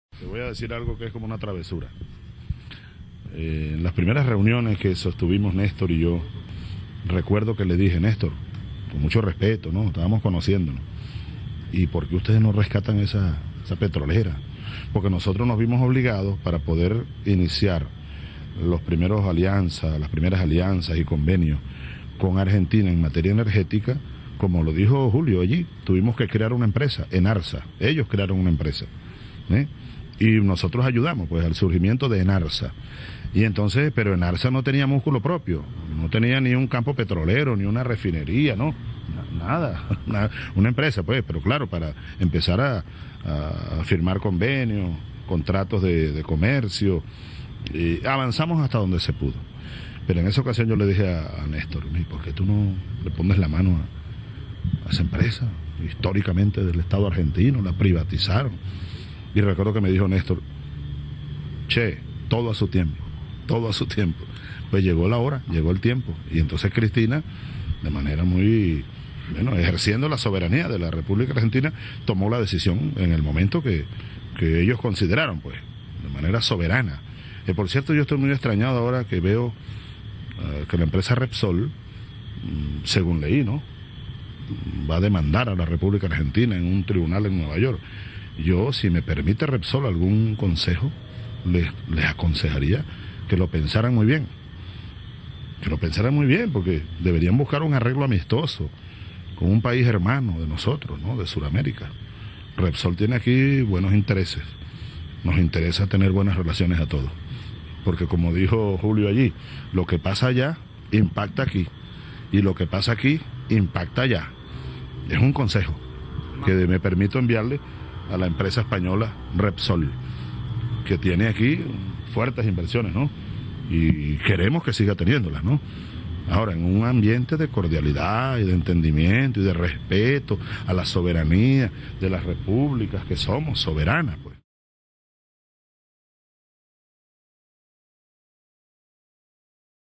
Chávez y Kirchner hablaron sobre la recuperación de YPF
El presidente de Venezuela Hugo Chávez reveló que en sus primeros encuentros con el ex presidente argentino Néstor Kirchner hablaron sobre la  posibilidad de que nuestro país recupere la soberanía sobre la empresa YPF.
"Yo recuerdo claramente que cuando le consulté respetuosamente si no existía esa posibilidad, Néstor me respondió 'che, todo a su tiempo, todo a su tiempo'. Y ese tiempo llegó" afirmó Chávez en diálogo con la prensa venezolana en la puerta del Palacio Miraflores, minutos después de haber mantenido una reunión con el ministro de Planificación Julio De Vido con quien trazaron los lineamientos generales para el trabajo conjunto de la petrolera estatal argentina y PDVSA.
El mandatario, minutos antes, confirmó que el próximo 31 de julio estará presente junto a la presidenta Cristina Fernández en la ceremonia que se realizará en Brasilia, para formalizar la incorporación plena de Venezuela al Mercosur.